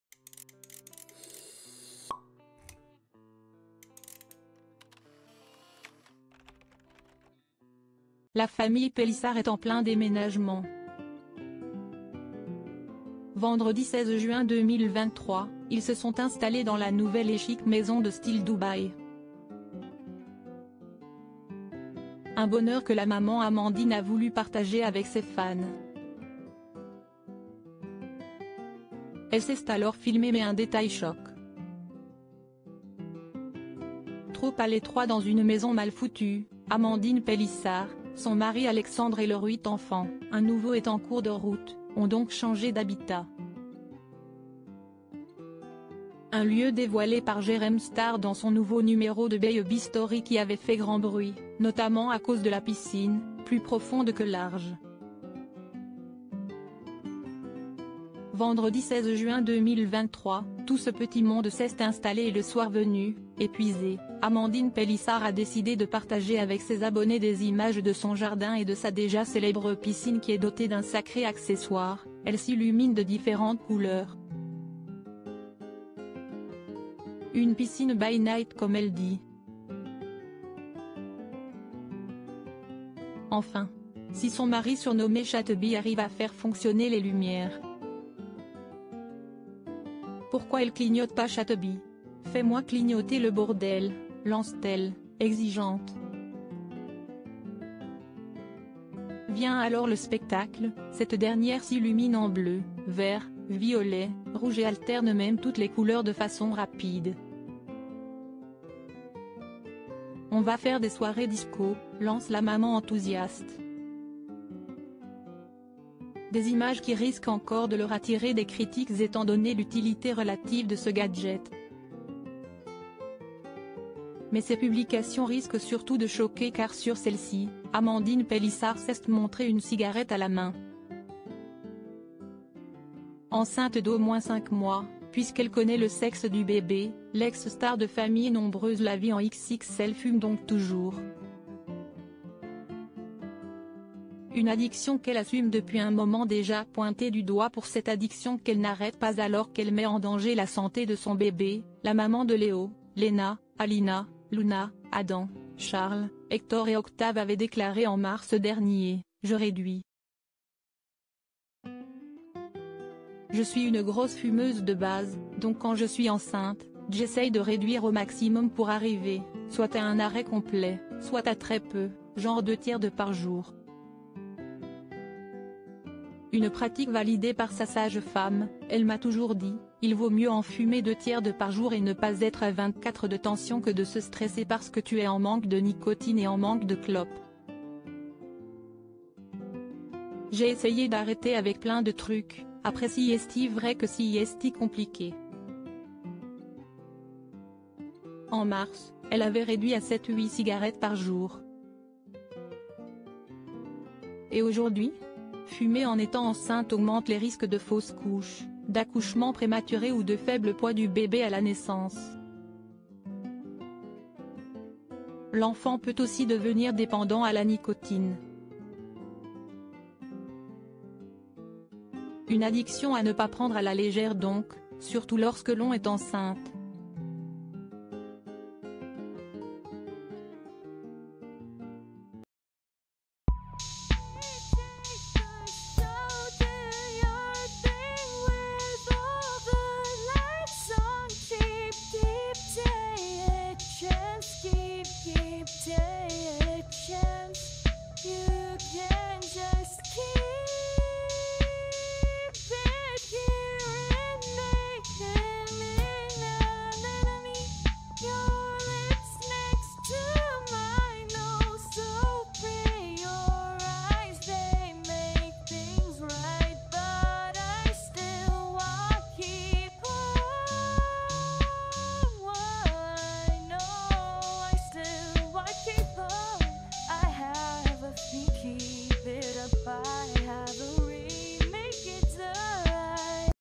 Amandine Pellissard, enceinte, se filme alors qu'elle s'adonne à une addiction dangereuse pour son b
Amandine Pellissard, enceinte, se filme alors qu'elle s'adonne à une addiction dangereuse pour son bébé